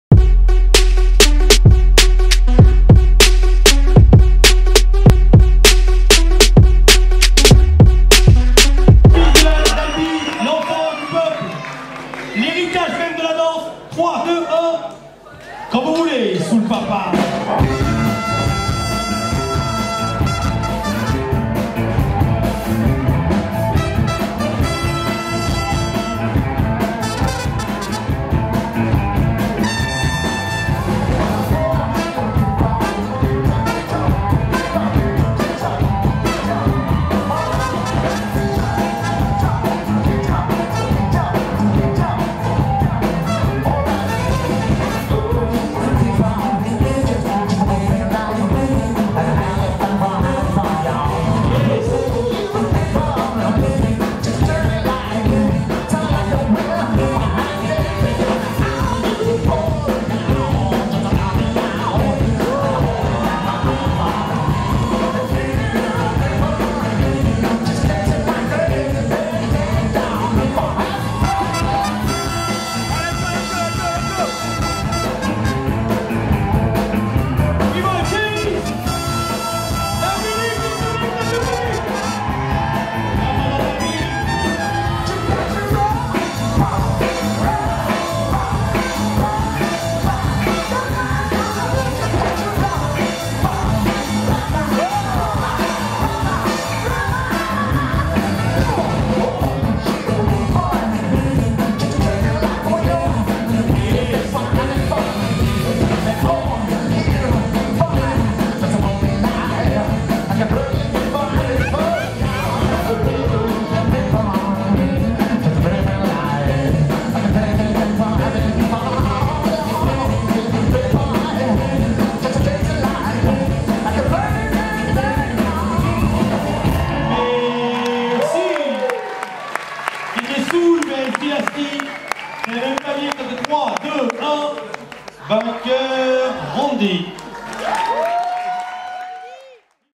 RANDY vs JEYZ │BREAK │ ALL 4 ALL BATTLE EUROPEEN 2017
DANCERS: Randy vs Jeyz
JUDGES: Walid, Pascal Blaise, Rickysoul
DJ'S: Tajmahal & Soul Papaz

All 4 All Battle Européen 2017
France, Albi